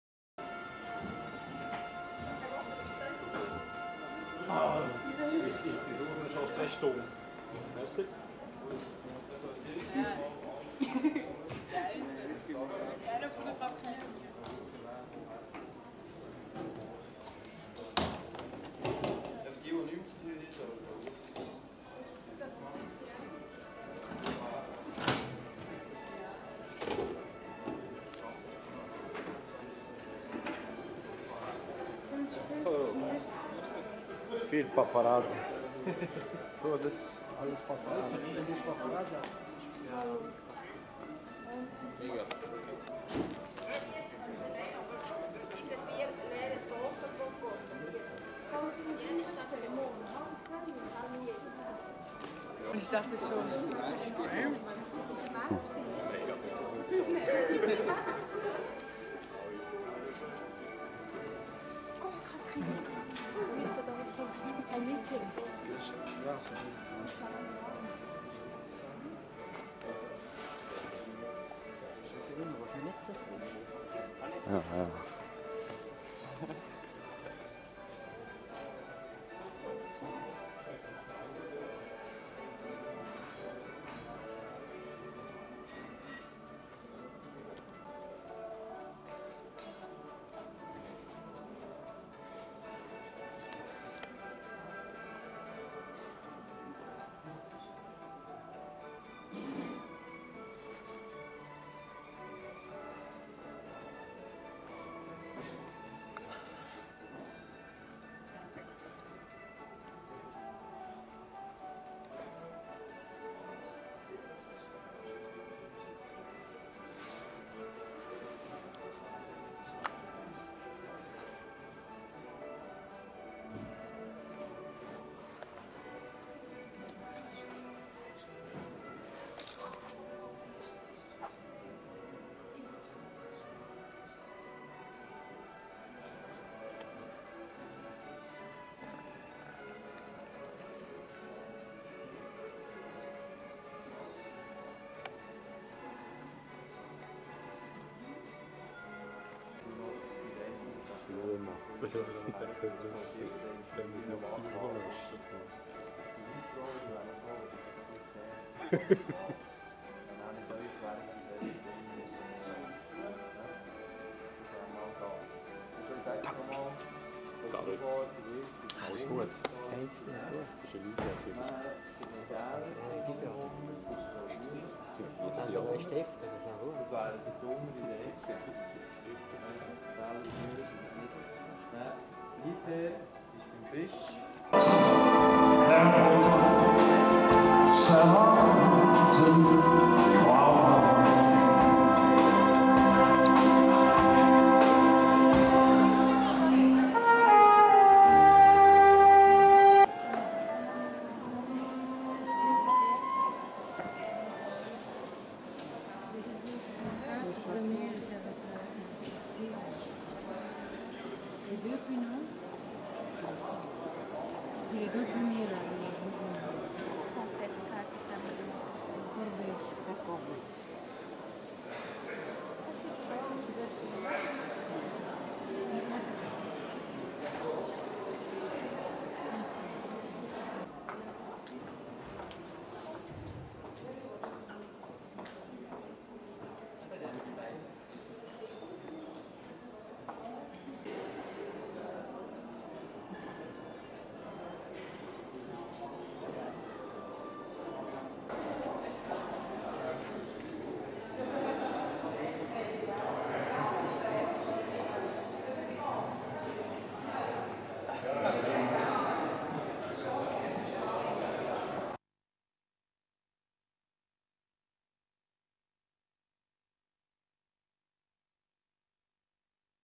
sommerball 2000 interlaken
galabuffet der küchenbrigade des victoria jungfrau